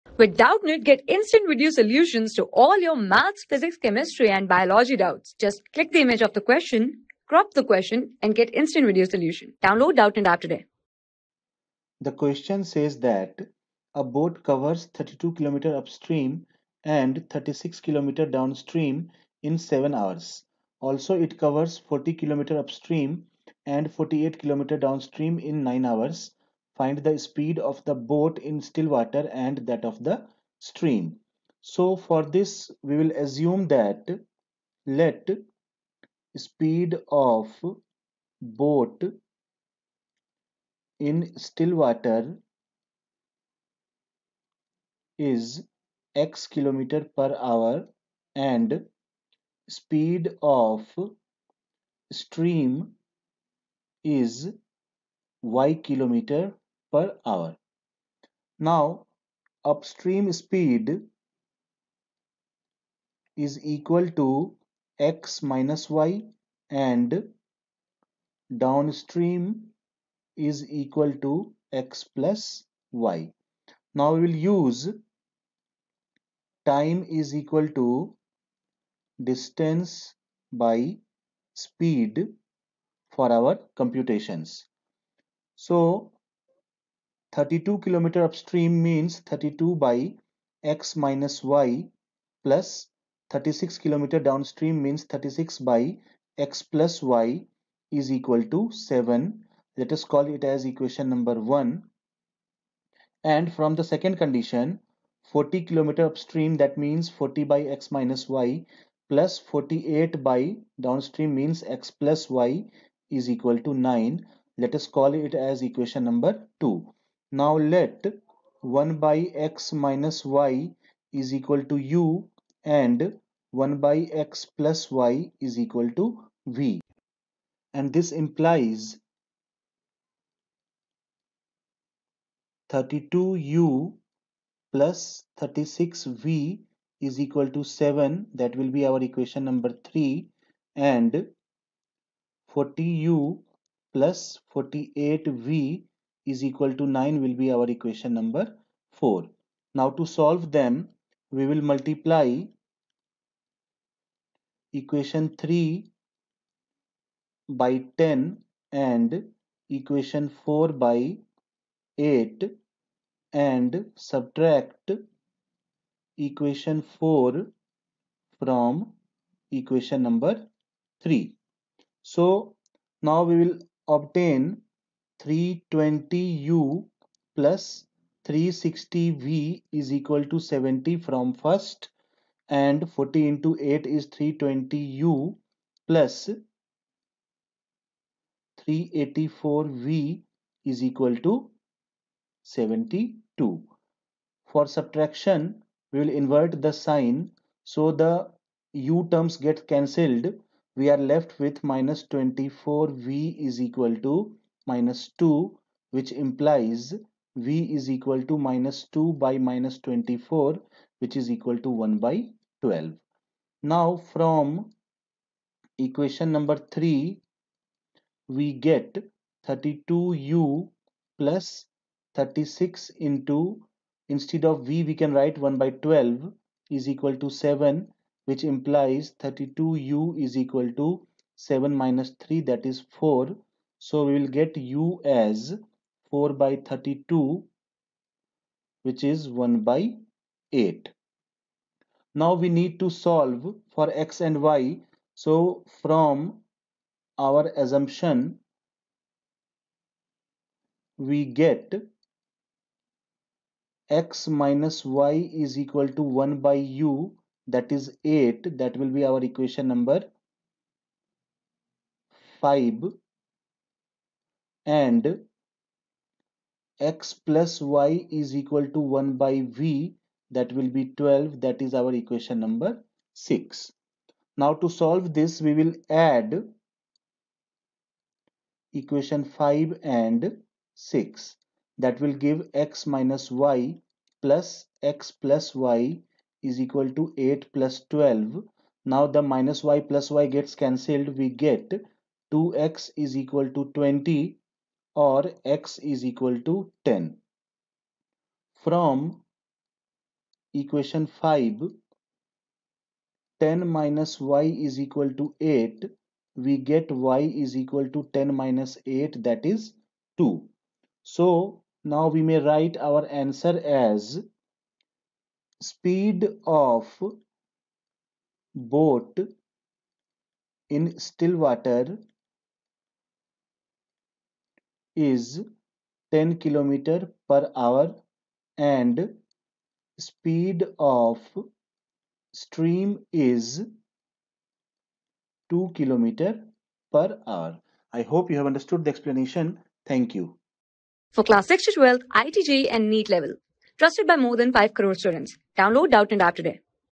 A boat covers 32km upstream and 36 km downstream in 7 hours. Also, it\r\ncovers 40 km upstream a...
A boat covers 32km upstream and 36 km downstream in 7 hours. Also, it\r\ncovers 40 km upstream and 48km downstream in 9 hours. Find the speed of the\r\nboat in still water and that of the stream.
Class: 10
Subject: MATHS
Chapter: PAIR OF LINEAR EQUATIONS IN TWO VARIABLES
Board:CBSE

👉 Have Any Query? Ask Us.